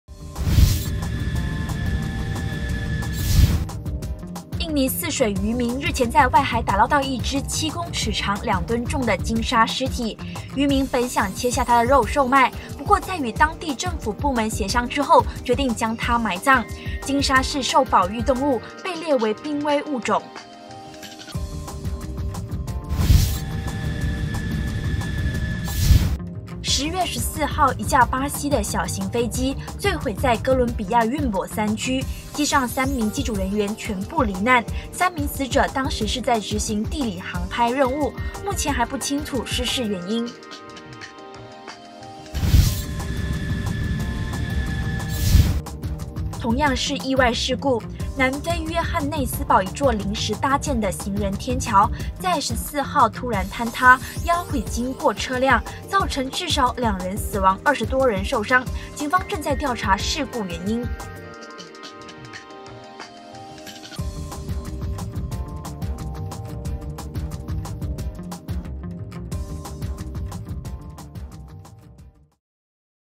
哥倫比亞·小型飛機墜毀3死
哥倫比亞·小型飛機墜毀3死；南非·行人天橋倒塌20多人死傷；印尼·漁民撈起2噸重鲸鲨屍體，來看看世界各地發生了什麼事吧！